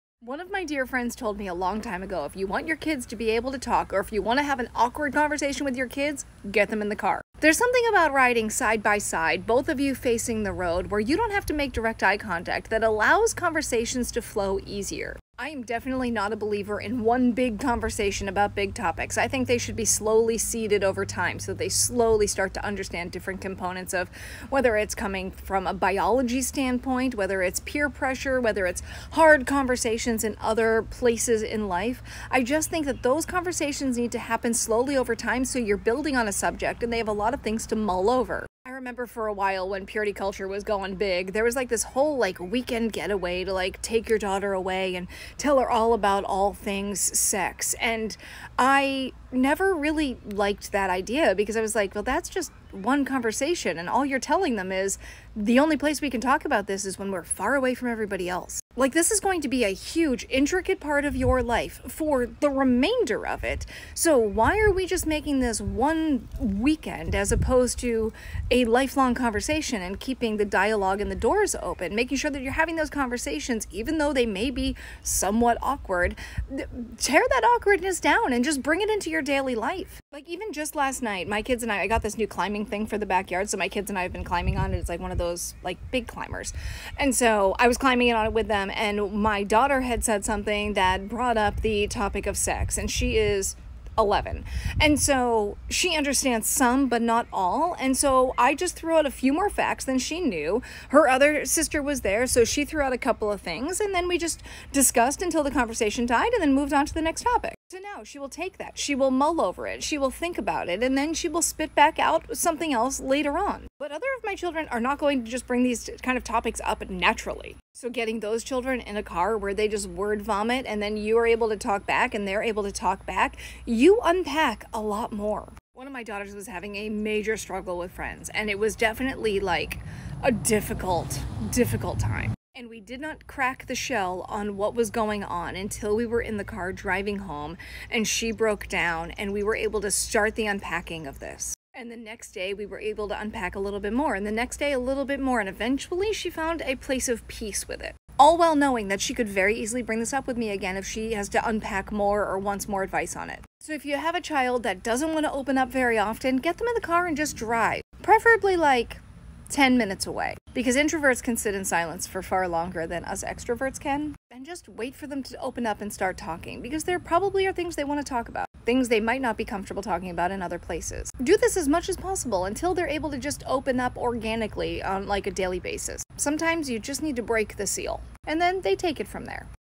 Parenting Secrets to Easily Handling Uncomfortable Talks with Kids
In this video, I'm going to share with you a secret to easily handling uncomfortable talks with kids. This is a technique that has helped me tremendously when it comes to talking to my kids about sensitive topics.

Most parents find themselves struggling to talk to their kids about difficult topics. This video will teach you a secret method that makes conversations with your kids much more comfortable and easy. With this strategy, you'll be able to handle difficult conversations with your kids in a calm and confident manner!